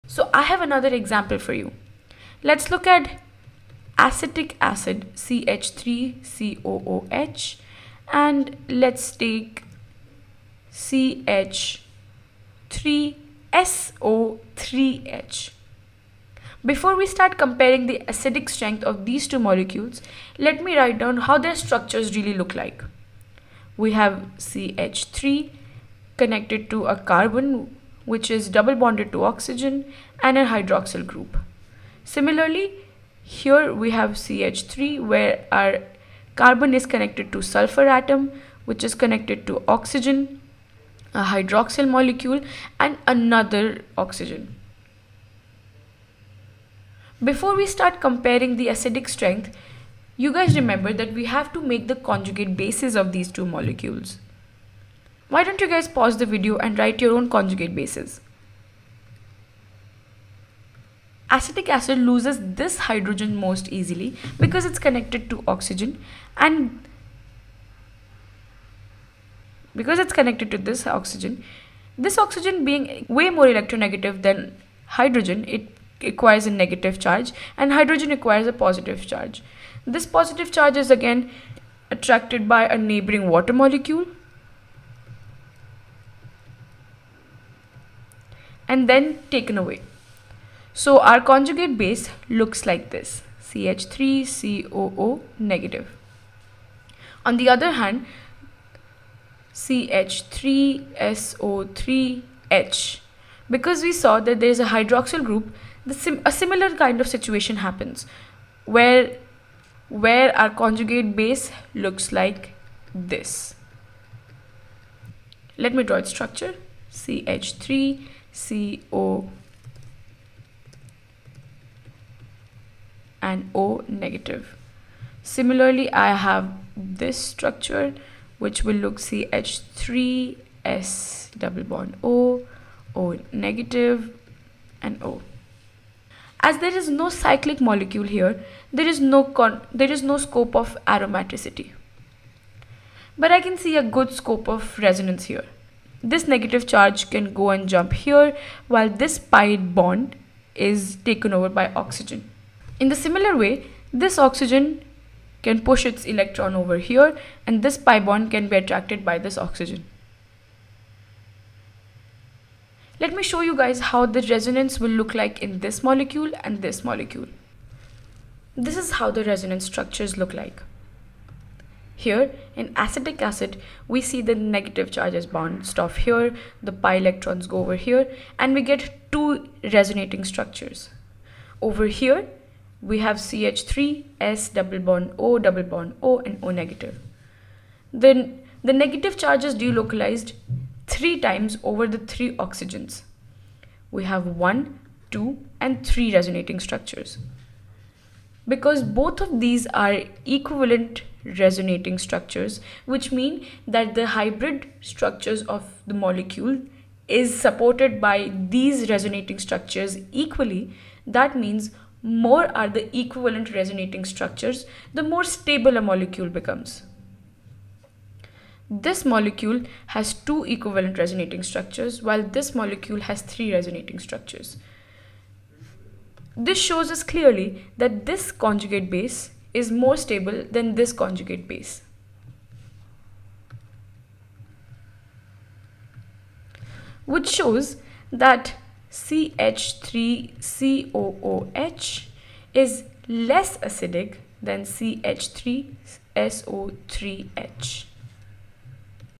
Using electronic effects to compare acidic strength - Resonance |Chemistry | Khan Academy
Check out more videos and exercises on “Acidic and basic strength”

To get you fully ready for your exam and help you fall in love with “Organic chemistry”, find the complete bank of exercises and videos for “organic chemistry essentials”